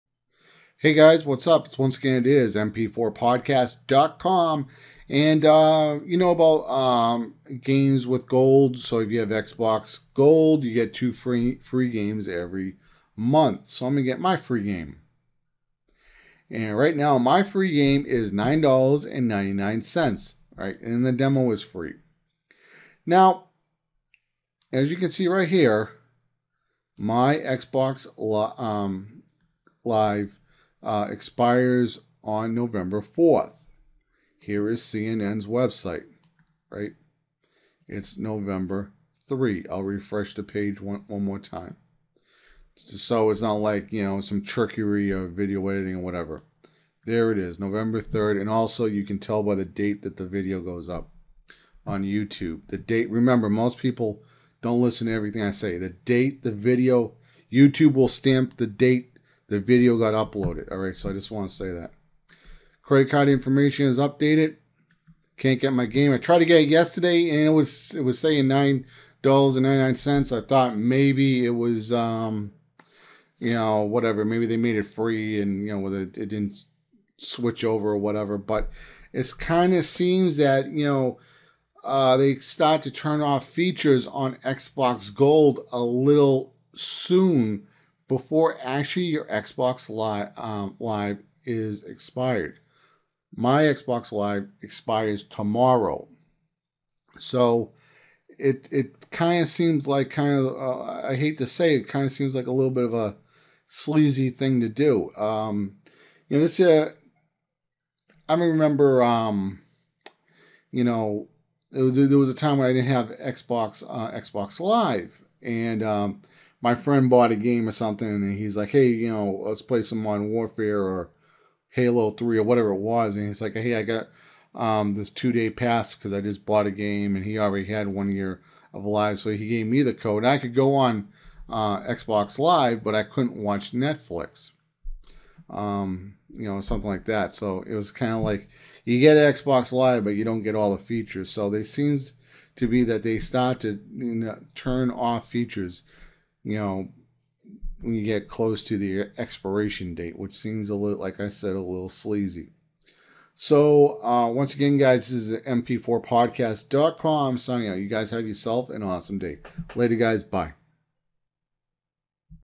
Microsoft not giving "Games with GOLD" if your Gold is about to expire soon
Microsoft please fix this.